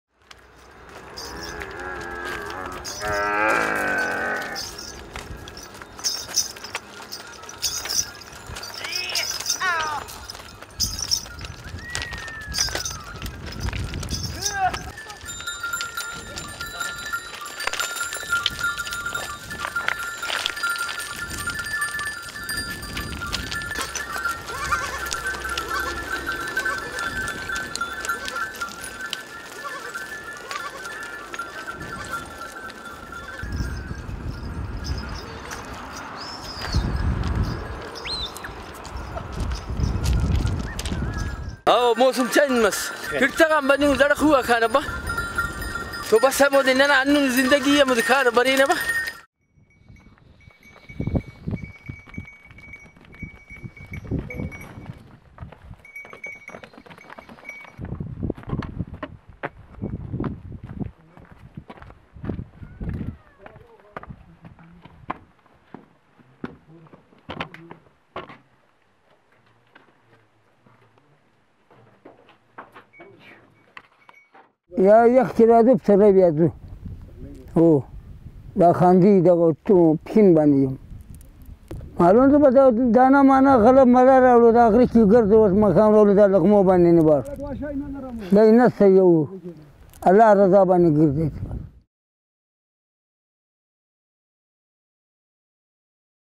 پہاڑوں میں غار، بلوچستان کے خانہ بدوش
سردی کی لہر آتے ہی خانہ بدوش شمالی بلوچستان کے سرد علاقوں سے گرم علاقوں کی جانب کوچ کرتے ہیں۔ وہ جہاں جاتے ہیں وہاں انہیں اپنے لیے پانی اور اناج کا بندوبست کرنا پڑتا ہے۔ ان کے بقول، ’’ہماری پوری زندگی سفر کرتے گزرتی ہے‘‘۔ رپورٹ دیکھئے۔